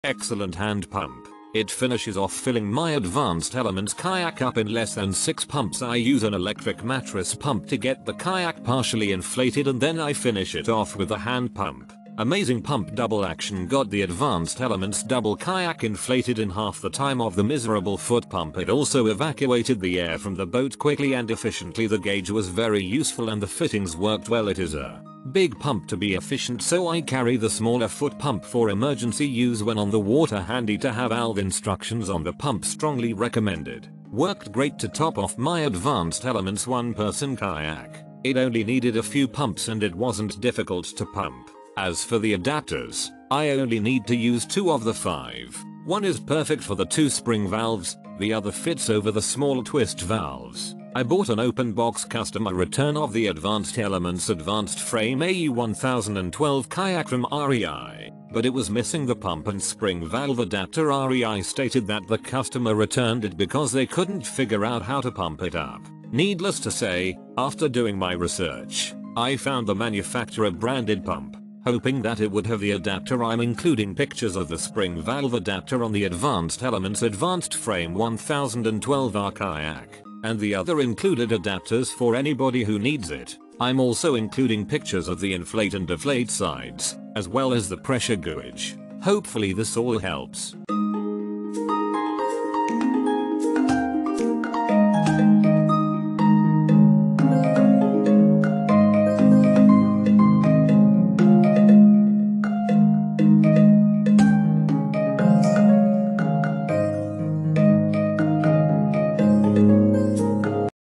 ADVANCED ELEMENTS Double Action Pump w//Pressure Gauge review
Main Features:
by entering your model number.. It is durable. High flow inflation. It is made of multiple adaptors. This high flow pump inflates and deflates. Designed with a pressure gauge